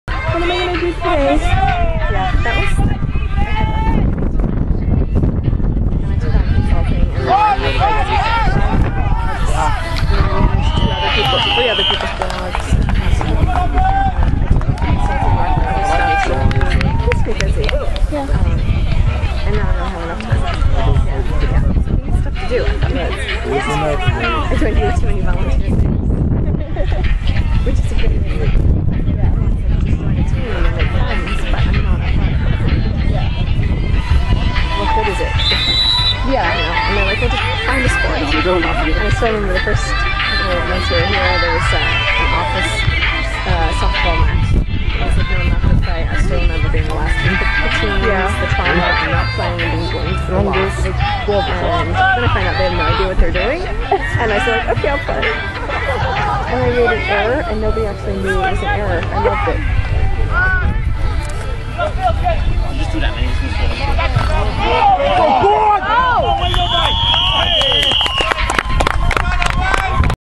Cork_admirals_4th_July_2010_blarneybowl_IAFL.MP4
Cork Admirals on 4th of july 2010 Blarney Bowl vs the Dublin Dragons in the Irish American Football League